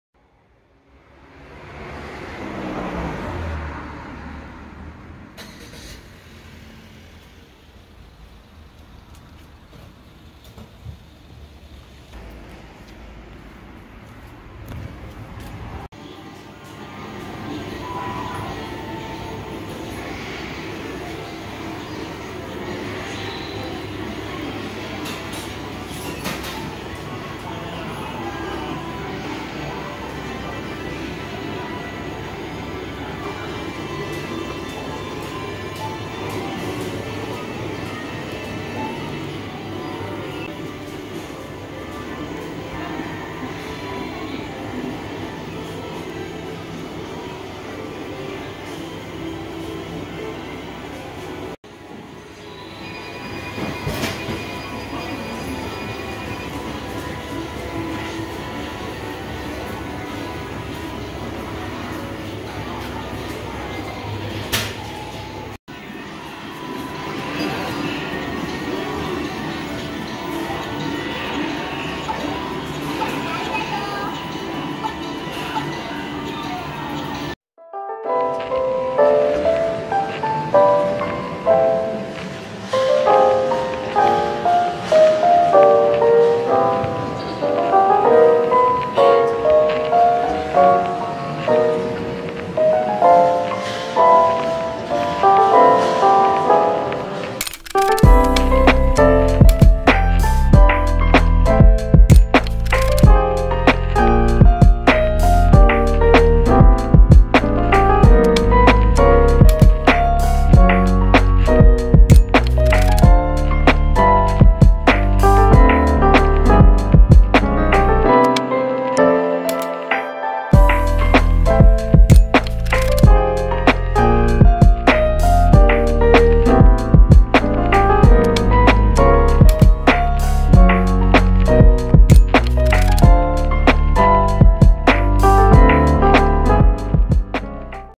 【新潟市北区の雄】ポピーとよさか 2023年11月 夜7時【コインスナック】
新潟市北区、旧豊栄の雄。
レトロ自販機、レトロゲームといった、そのまんま純度100％の昭和レトロ。